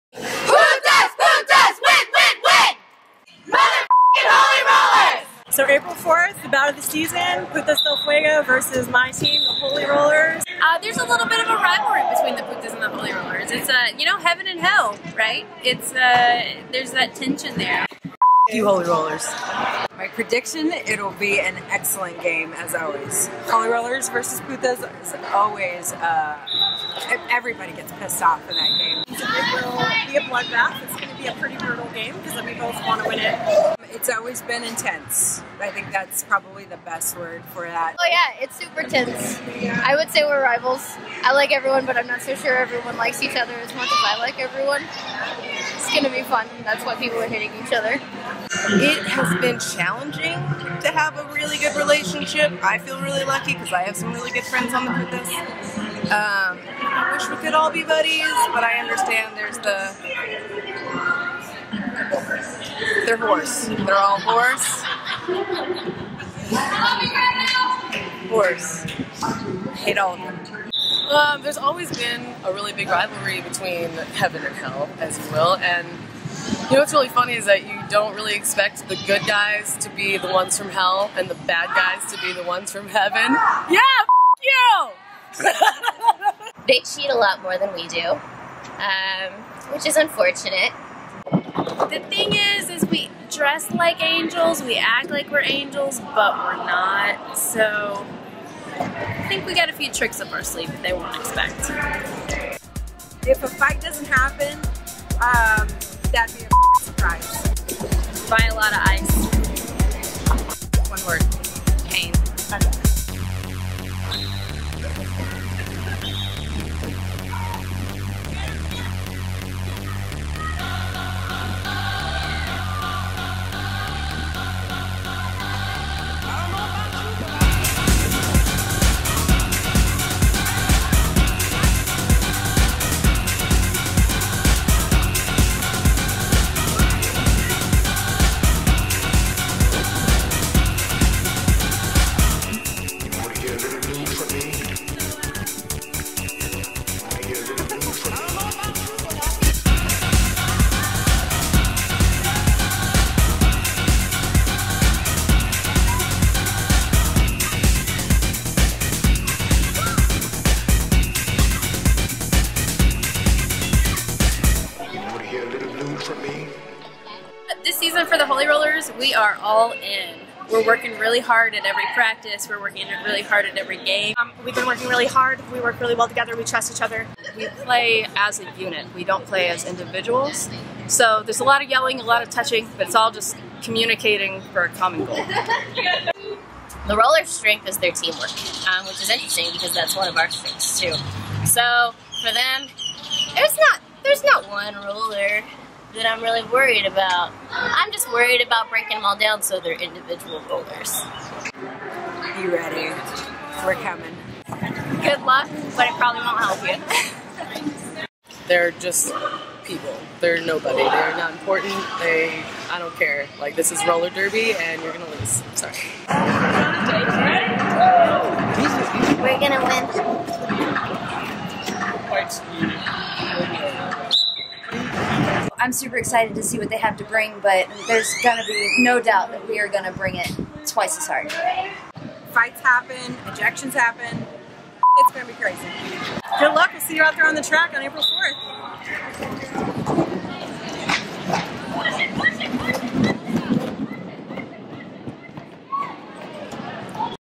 Putas vs Holy Rollers - 4/4/15
The Holy Rollers have come down from Heaven to send Las Putas back to Hell, but will the fuego burn too hot for them? Be at the Palmer Events Center April 4th to find out.

$1 from every ticket sold goes to benefit to help provide domestic violence assistance to the deaf and hard-of-hearing community